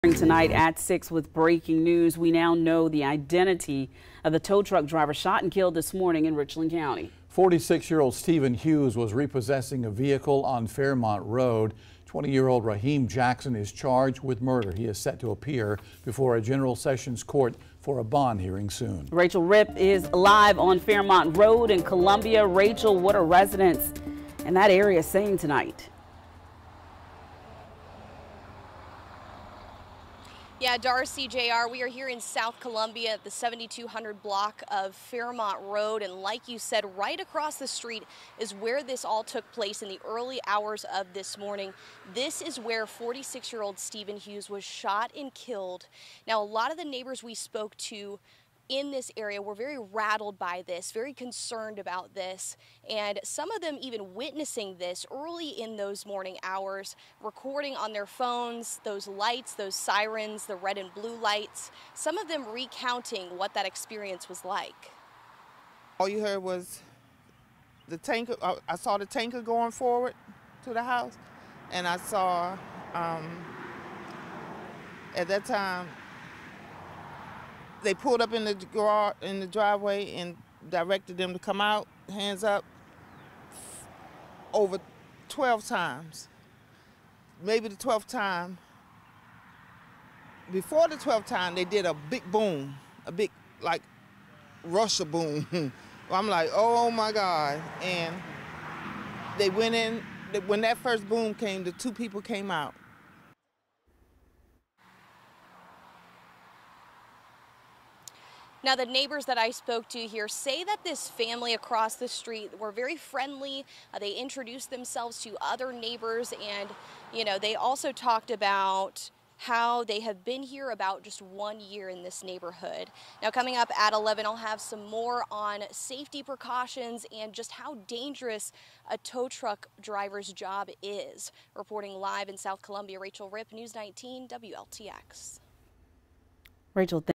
Man arrested in deadly shooting, victim identified
Raheem Jackson, 20 is charged with murder of Steven Hughes, 46 was was repossessing a car.